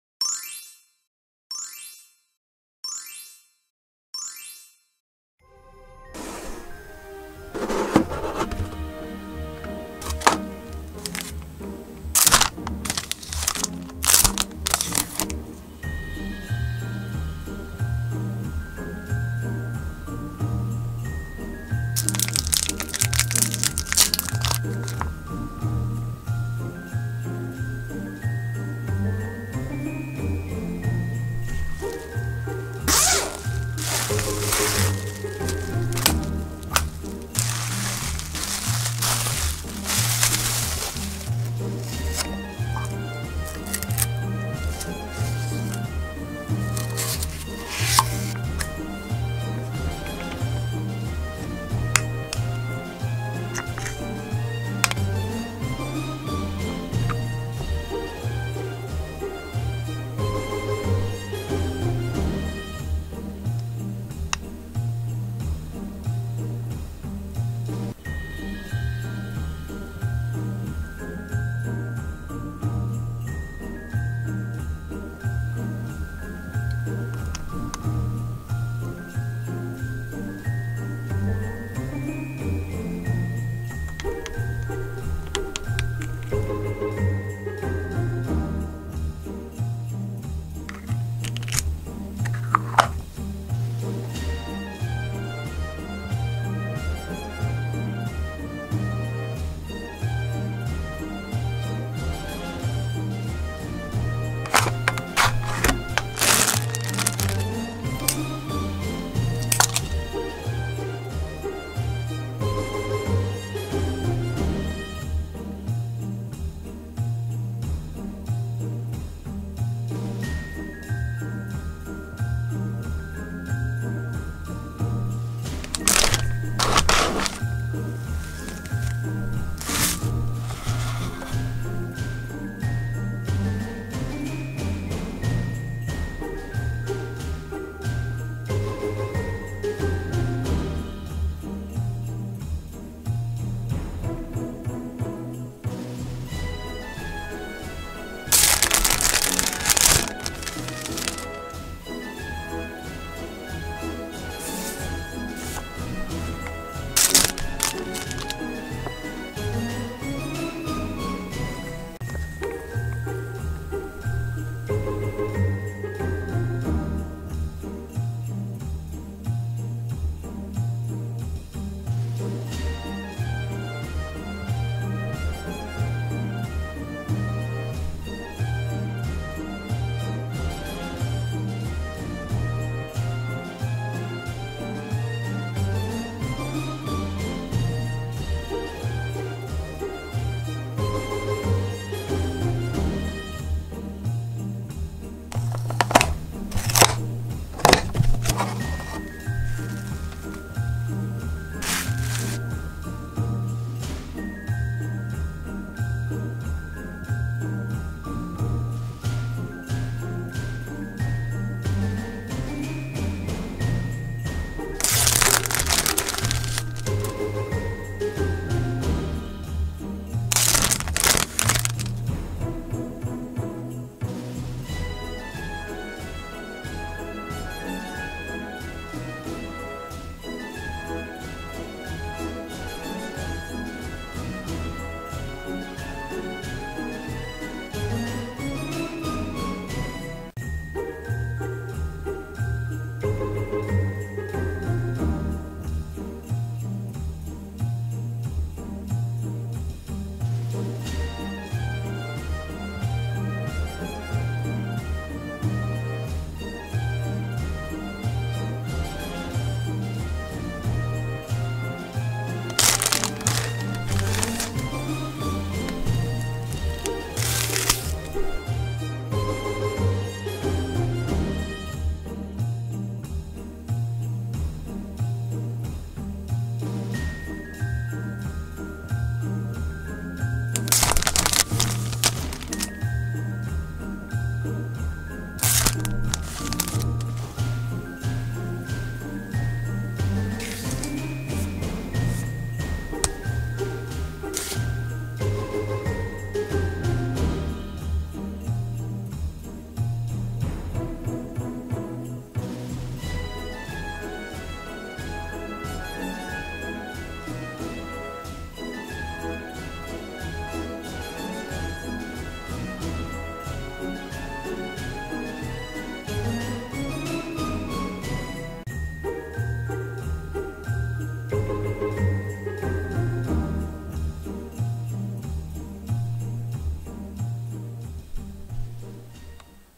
★구체관절인형 카날코드 치비나비[USD]개봉후기★Ball Jointed Doll CanalCode Chibi Nabi Unboxing/BJD/asmr
*만 14세 이상 구매 가능
 놀이용 완구가 아니며, 15세 미만 어린이가 사용 할 수 없습니다.
*Available for purchase over 14 years of age
 This is not a play toy and is not intended for use by children under 15 years of age.